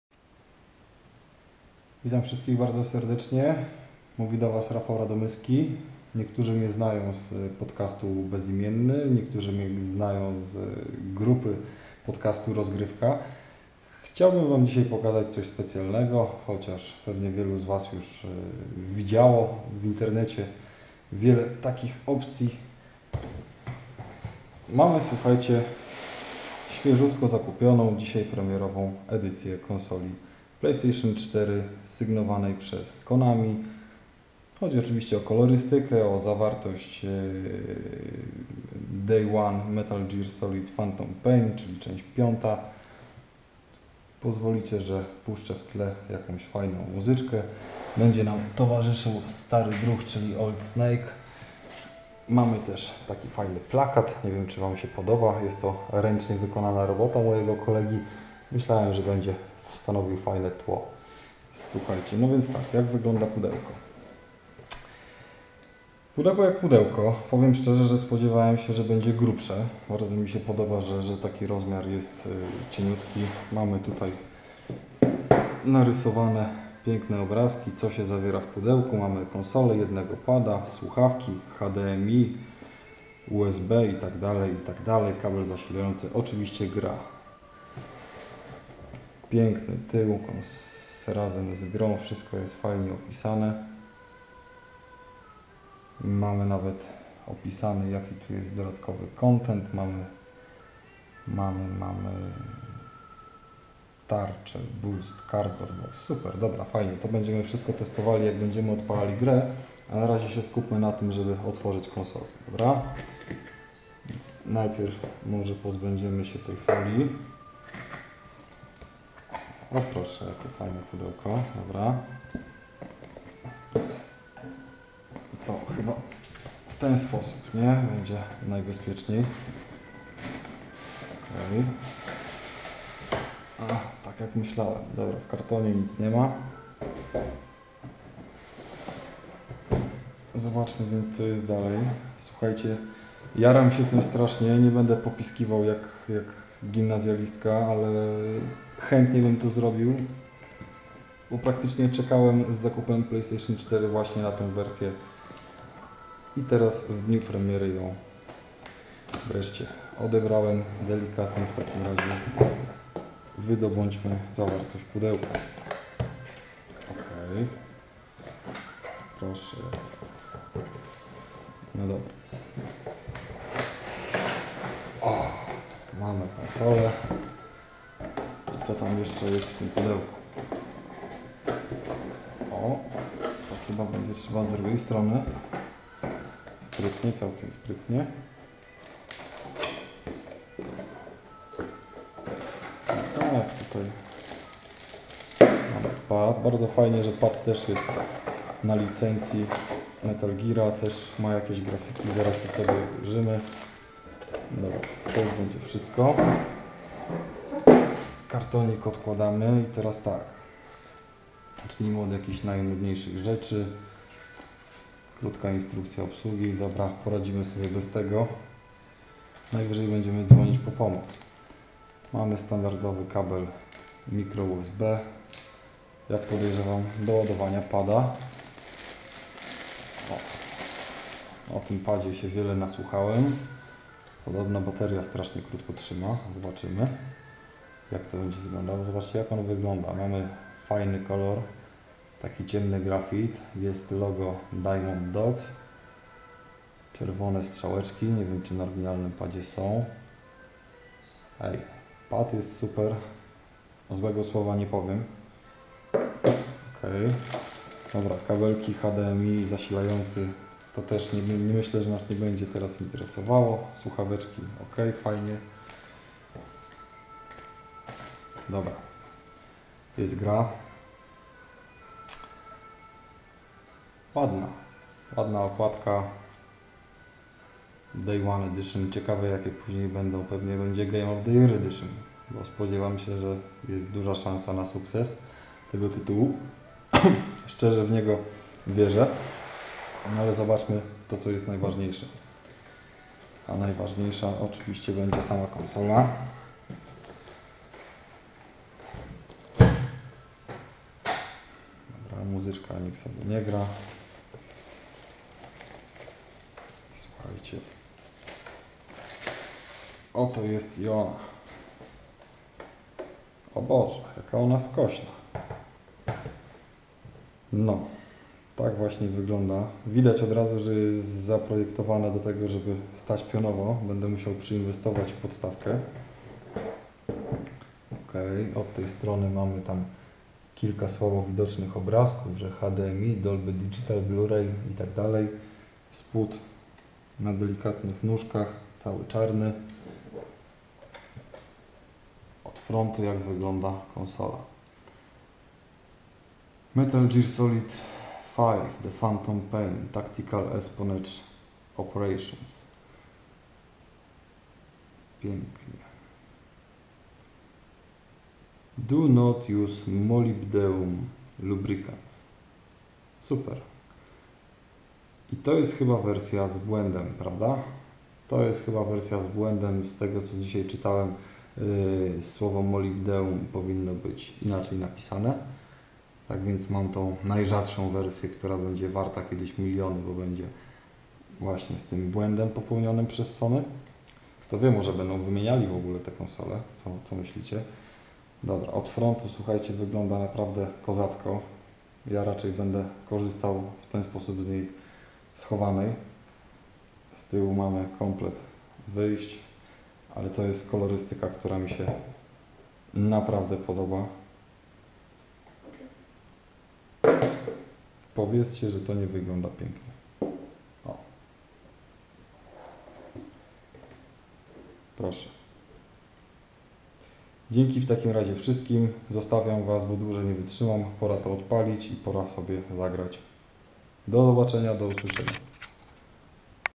Playstation 4 - The Phantom Pain Edition Unbox
Piękne rozpakowanie, pięknej konsoli w barwach Metal Gear Solid V The Phantom Pain. Możecie poczuć się jakbyście sami otwierali tą limitowaną konsole Sony . Miłego oglądania!